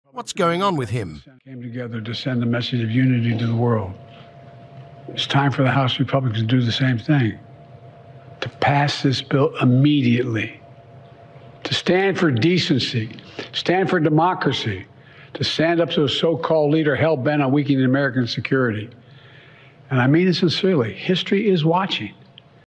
Republicans and Democrats Unite for Unity! It's Your Turn, House Republicans!
In a rare show of bipartisanship, Republicans and Democrats come together to advocate for unity and cooperation in the political arena. As the country grapples with pressing issues and challenges, leaders from both sides of the aisle join forces to prioritize the greater good over partisan divides. Now, the spotlight turns to House Republicans as the nation eagerly awaits their response. Will they heed the call for unity and collaboration, or will political differences continue to dominate the discourse? Unity Bipartisanship PoliticalCooperation HouseRepublicans UnitedStates news trump donaldtrump 🇺🇸🤝